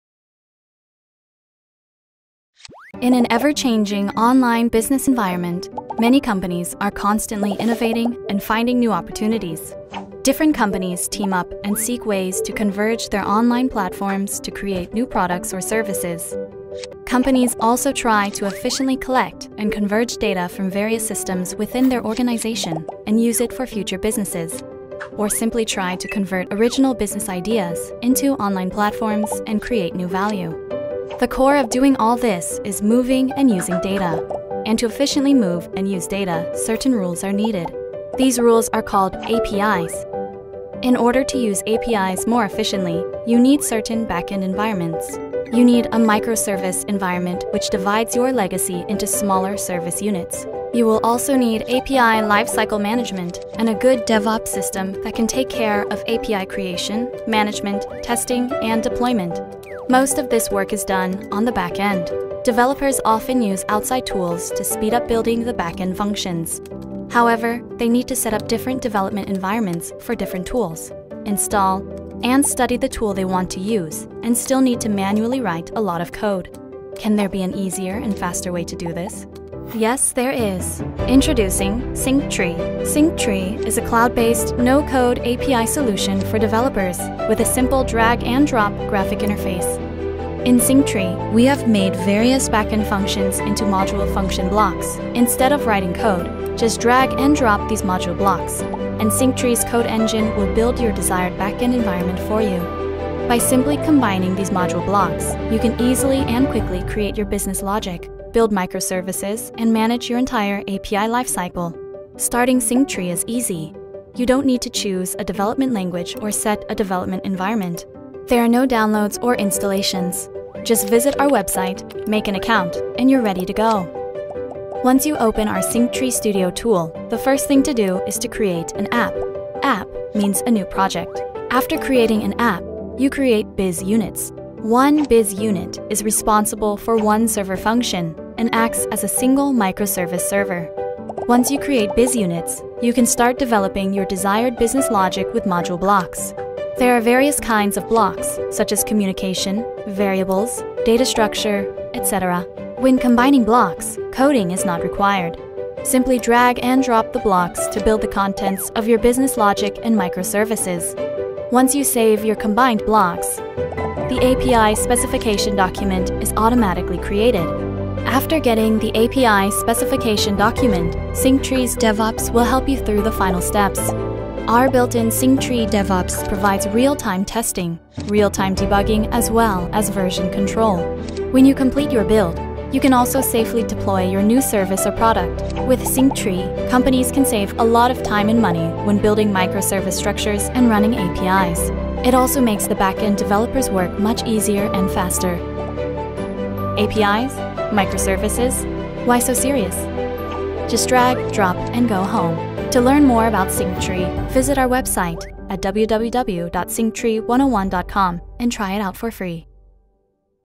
Introduction to SyncTree - No code API solution [KOR]
SyncTree는 코딩 없이 모듈화 된 다양한 기능 블록을 ‘드래그 앤 드롭’으로 결합하여 비즈니스 로직 생성부터 서비스의 Microservice구현 그리고 API의 생성, 관리, 테스트, 배포, DevOps까지의 모든 작업을 한 개의 Tool에서 할 수 있는 ‘No- Code API Solution’ 입니다.